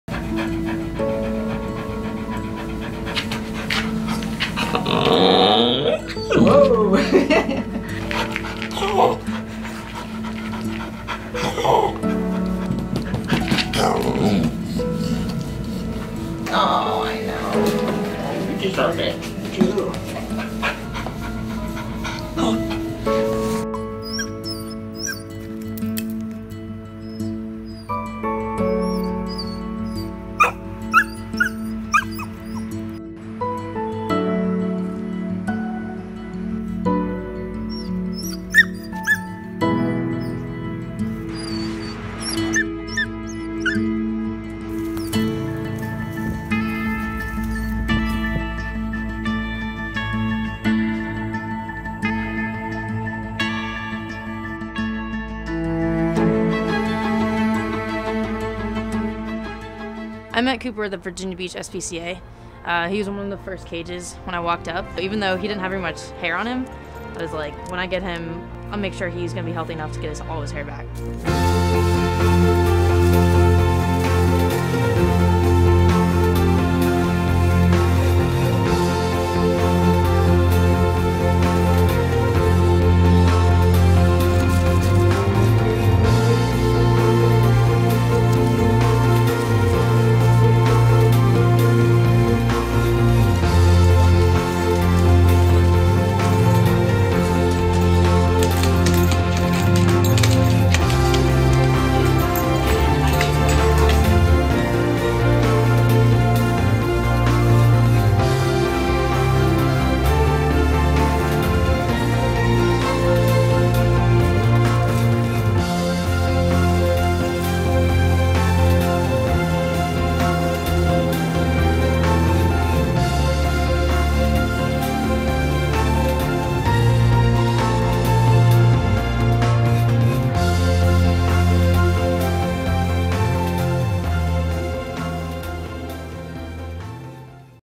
Six Years on a Chain Couldn't Keep Him Down
WE’RE HIRING: You can help lead PETA’s efforts to affect change for “outdoor dogs” by applying to be our Community Outreach Liaison.

For the first six years of his life, Cooper the dog was chained up, outside around-the-clock. He couldn’t run and play, he was desperately lonely, bored, and had no relief from a terrible flea allergy that caused him to chew off most of the fur on his back legs. But everything changed when his owner finally gave him to PETA, and the next chapter of his life officially began.